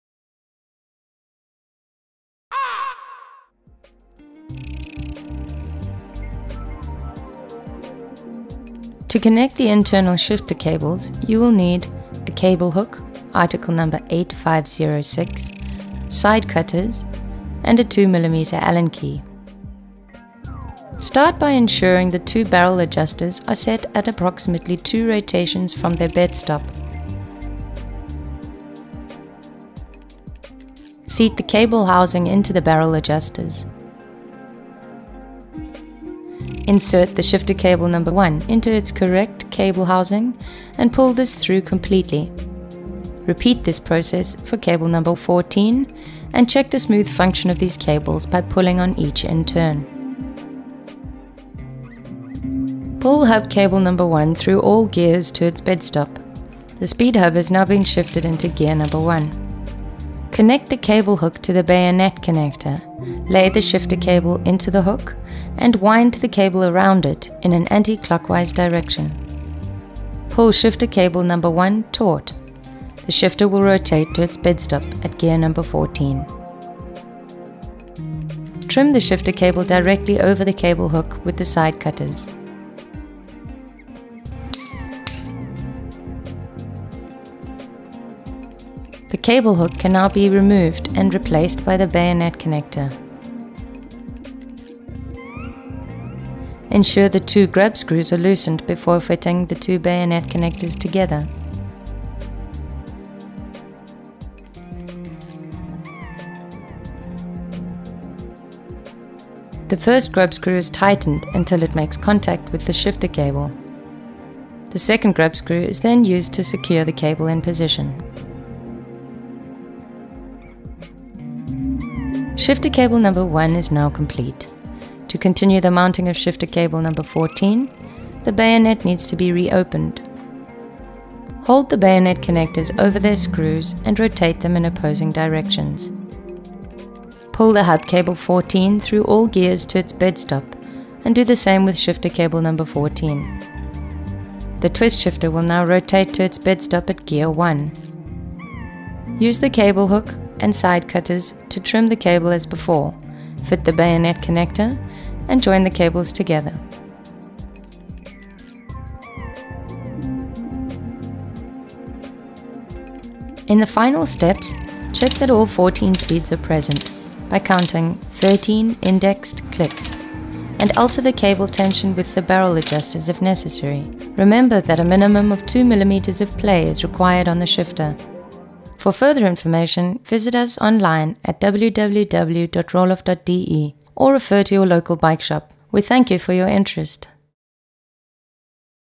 SPEEDHUB Workshop - Internal gear mech - cable replacement
To connect the internal shifter cables, you will need: a Cable Hook (Article #8506), side cutters and a 2mm Allen key.

Start by ensuring the two barrel adjusters are set at approximately 2 rotations from their bedstop.

Seat the cable housing into the barrel adjusters. Insert the shifter cable #1 into its correct cable housing and pull this through completely. Repeat this process for shifter cable #14 and check the smooth function of these cables by pulling on each in turn.

Pull hub cable #1 through all gears to its bedstop. The SPEEDHUB has now been shifted into gear #1. Connect the Cable hook to the bayonett connector, lay the shifter cable into the hook and wind the cable around it in an anticlockwise direction.

Pull Shifter cable #1 taut -- the shifter will rotate to its bedstop at gear #14.

Trim the Shifter cable directly over the cable hook with the side cutters.

The Cable Hook can now be removed and replaced by the Bayonett connector. Ensure the two grub screws are loosened before fitting the two bayonet connectors together.

The first grub screw is tightened until it makes contact with the shifter cable. The second grub screw is then used to secure the cable in position.

Shifter cable #1 is now complete. To continue the mounting of shifter cable #14, the bayonet needs to be reopened.  Hold the bayonet connectors over their screws and rotate them in opposing directions.

Pull the hub cable #14 through all gears to its bedstop and do the same with shifter cable #14. The twist shifter will now rotate to its bedstop at gear #1. Use the cable hook and side cutters to trim the cable as before, fit the bayonet connector and join the cables together.

In the final steps, check that all 14 speeds are present - by counting 13 indexed 'clicks' - and alter the cable tension with the barrel adjusters if necessary. Remember that a minimum of 2mm of play is required on the shifter.